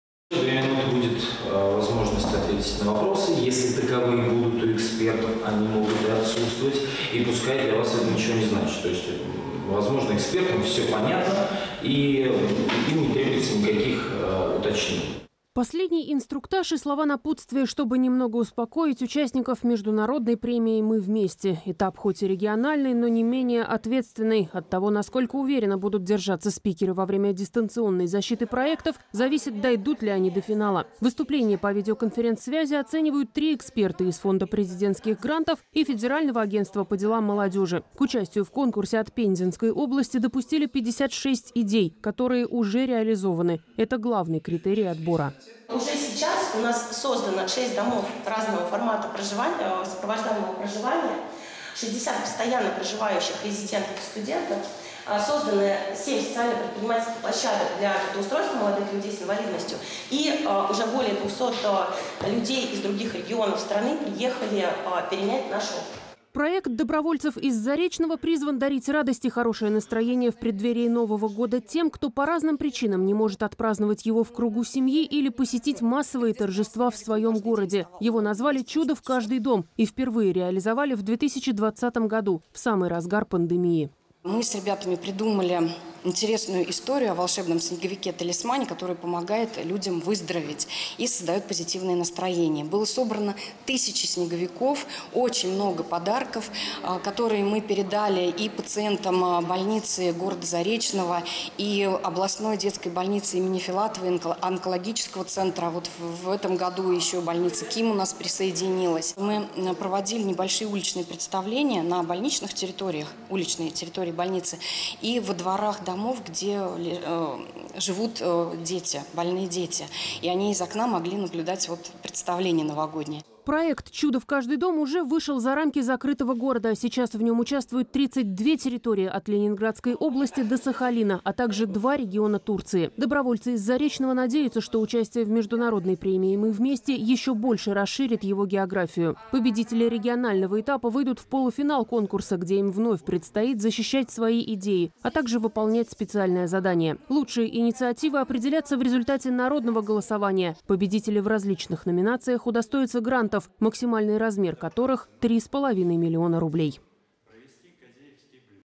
В Доме молодежи состоялся региональный этап международной премии «Мы вместе»
К участию в нем допустили 56 социальных проектов, разработанных волонтерами, предпринимателями и некоммерческими организациями Пензы и области. Претендентам предстояло отстоять свои идеи перед федеральными экспертами. Онлайн-защита длилась два дня.